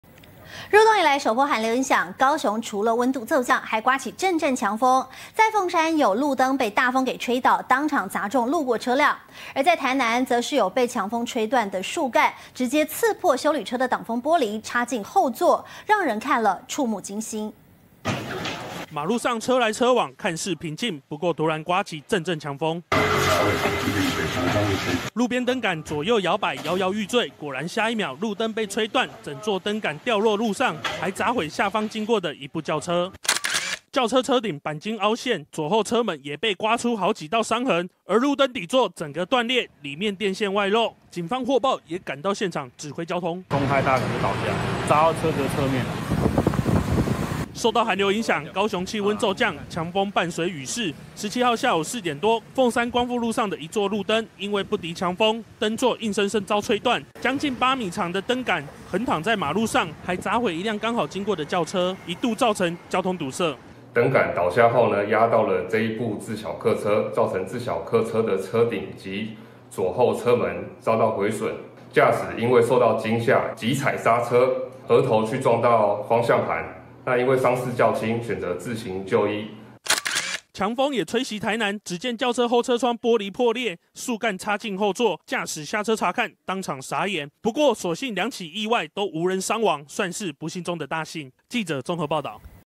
8米燈杆被吹斷橫躺路中 交通一度堵塞
受到入冬首波寒流影響，高雄除了氣溫驟降，也颳起陣陣強風，在鳳山就有路燈，遭大風吹倒，當場砸中路過轎車，而在台南，也有遭強風吹斷的樹幹，直接刺破休旅車擋風玻璃，插進車輛後座，讓人看了怵目驚心!